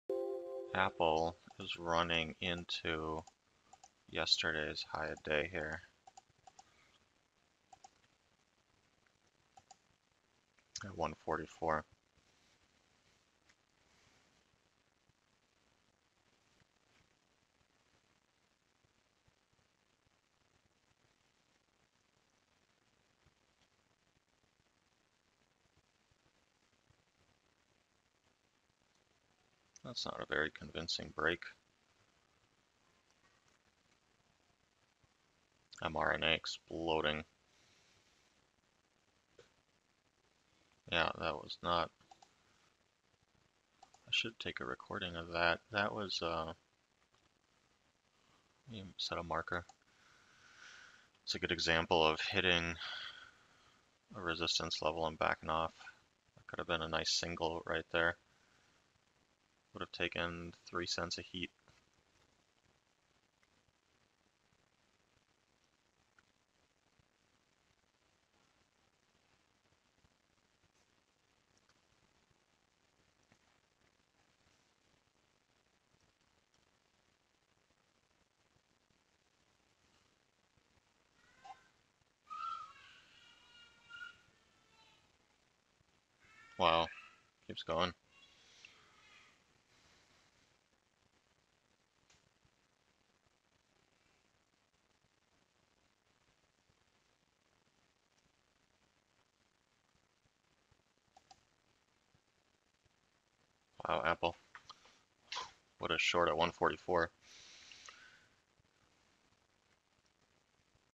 Study: AAPL Pop Into Resistance Previous HOD, Level Confirmed with Footprint
07/20/2021

On the open AAPL pops up into previous day's resistance at previous HOD 144.  Footprint chart shows seller absorbing at the level and buyers unwilling or unable to push higher. 1 pt drop follows.